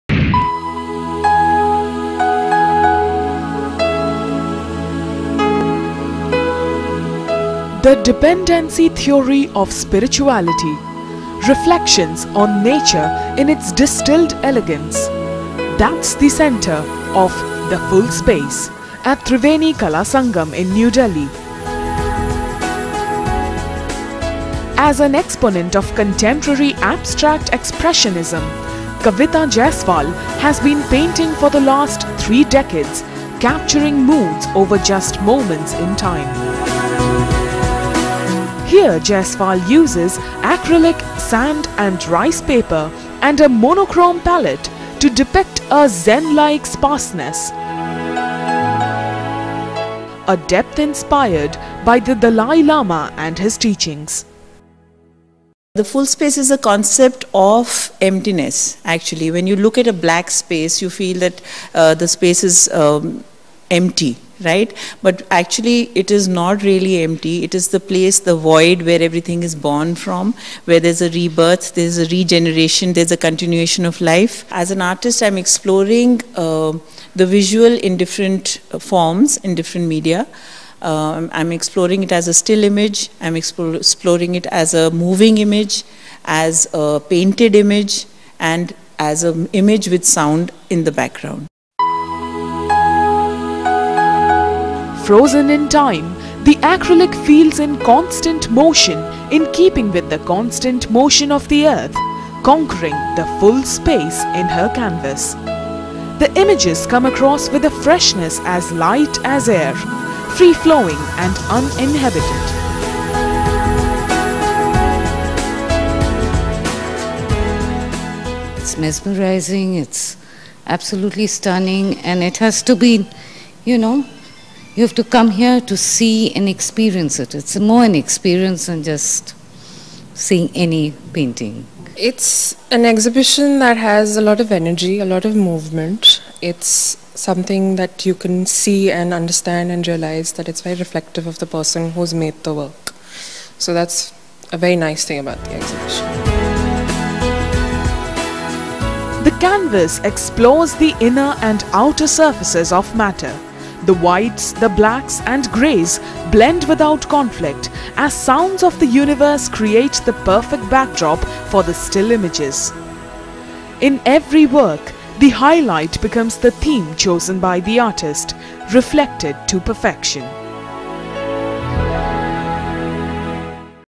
The Full Space
Kavita Jaiswal’s captures co-existence with her new show ‘The Full Space’